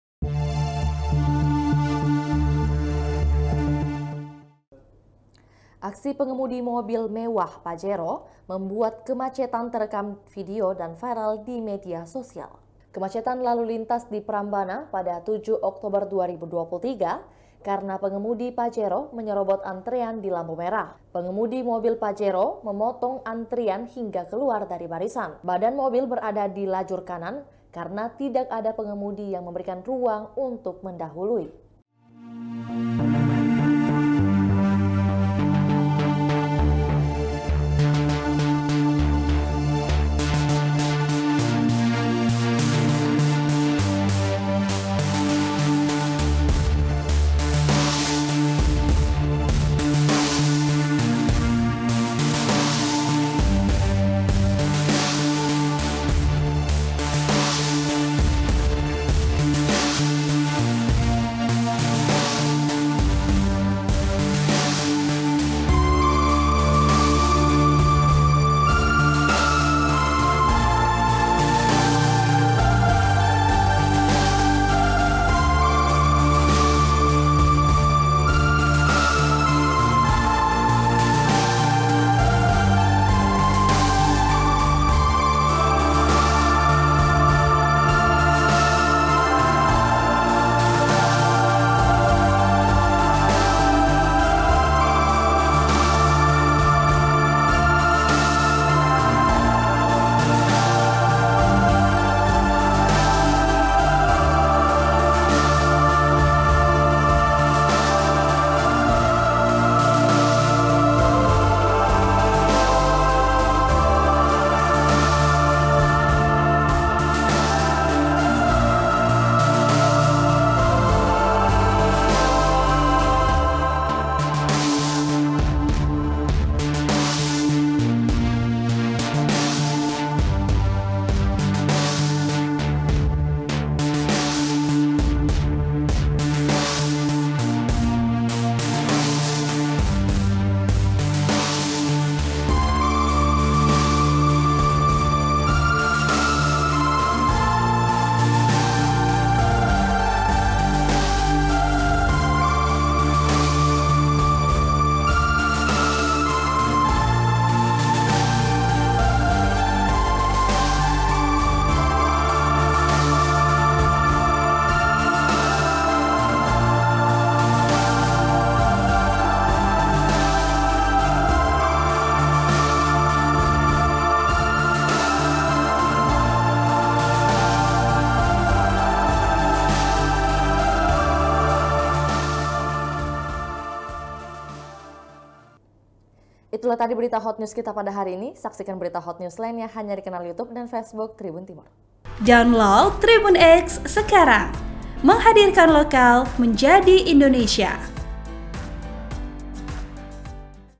Pengemudi Pajero Langgar Arus Hingga Tutup Jalan, Tak Mau Ngalah
Kemacetan lalu lintas di Prambana (7/10/2023) karena pengemudi Pajero menyerobot antrean di lampu merah.

Pengemudi mobil Pajero memotong antrian hingga keluar dari barisan.

Badan mobil berada di lajur kanan karena tidak pengemudi yang memberikan ruang untuk mendahului.

Naskah: Budi Kurniawan/Arny
Voice Over: Budi Kurniawan (Mahasiswa Magang Unismuh)

Host: Fiorena Jieretno
VP: Asriadi
Rica Monica Mahasiswa Magang IAIN Parepare
OP: Faiz
Narator: Arny